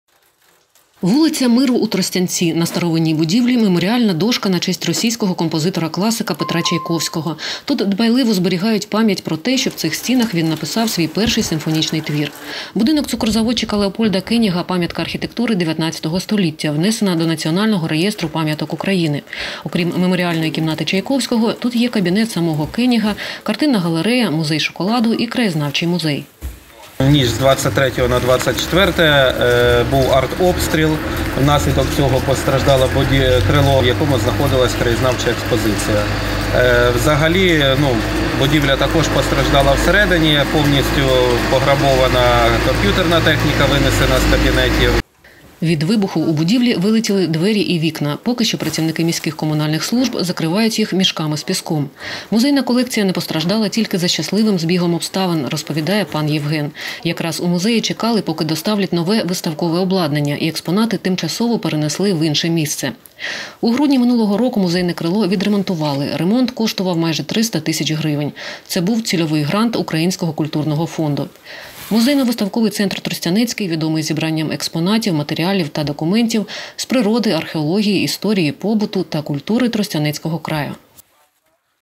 На Сумщині намагаються захистити садибу Кеніга
У Тростянці на Сумщині намагаються захистити будівлю 19 століття - садибу Леопольда Кеніга, де розташований Тростянецький краєзнавчий музей